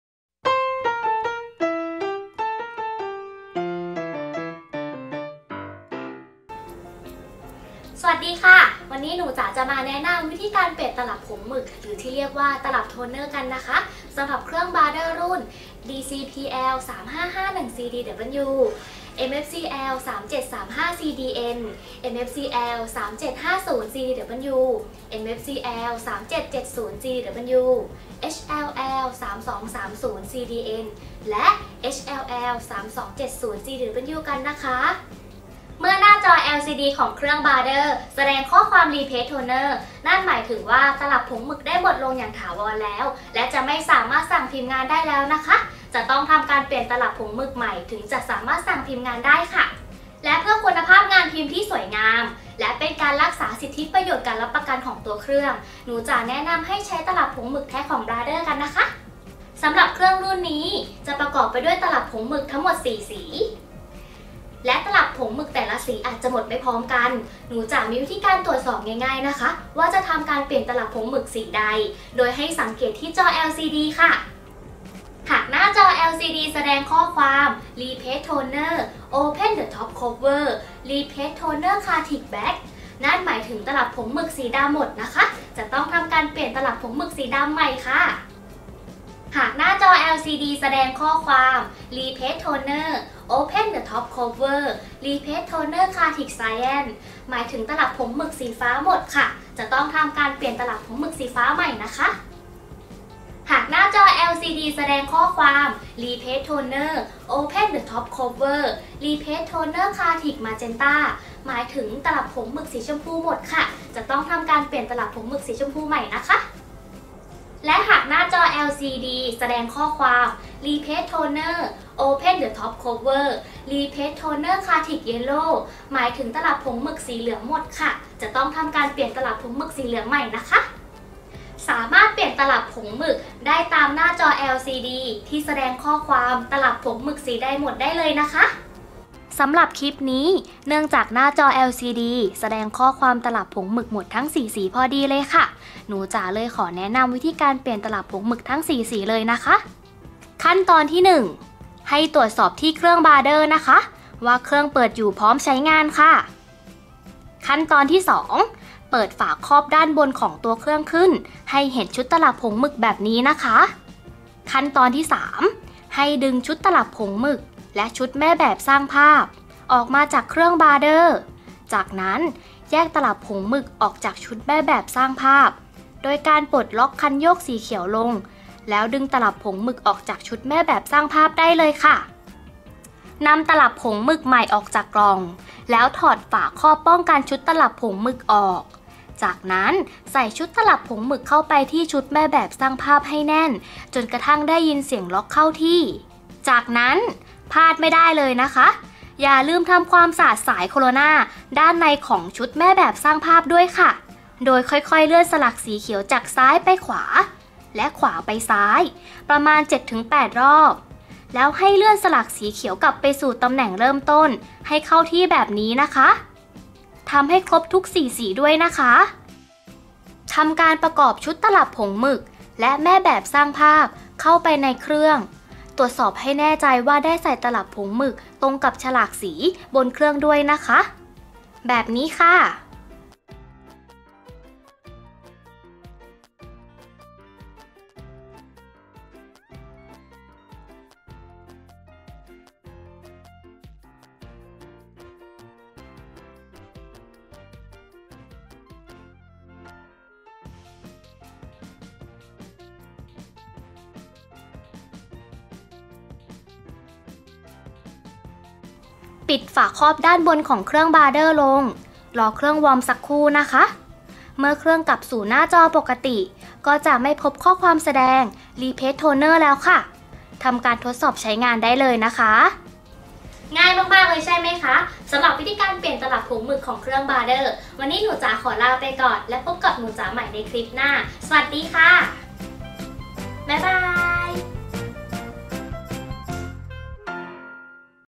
วิธีการเปลี่ยนตลับผงหมึกสำหรับเครื่องบราเดอร์เทคโนโลยีเลเซอร์สี LED
หนูจ๋าขอนำคลิปวีดีโอดี ดี มาฝากทุกท่านนะคะ เป็นคลิปวีดีโอวิธีการเปลี่ยนตลับผงหมึกสำหรับเครื่องบราเดอร์เทคโนโลยีเลเซอร์ LED ค่ะ เมื่อดูแล้วจะทำให้การเปลี่ยนตลับผงหมึกง่ายขึ้นอีกเยอะเลยค่า